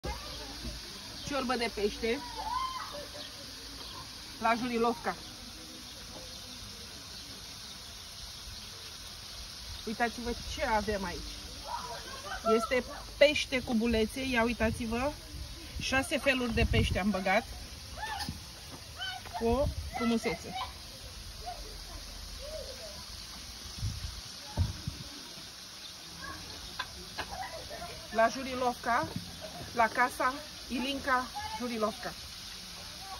Ciorba de peste ca la Jurilovca, la Casa Ilinca
Ciorba de peste ca la Jurilovca, la Casa Ilinca, facuita la ceaun, cu peste proaspat, prins dimineata. Adevarata ciorba de peste dobrogeana, asa cum o fac pescarii. Casa Ilinca este aici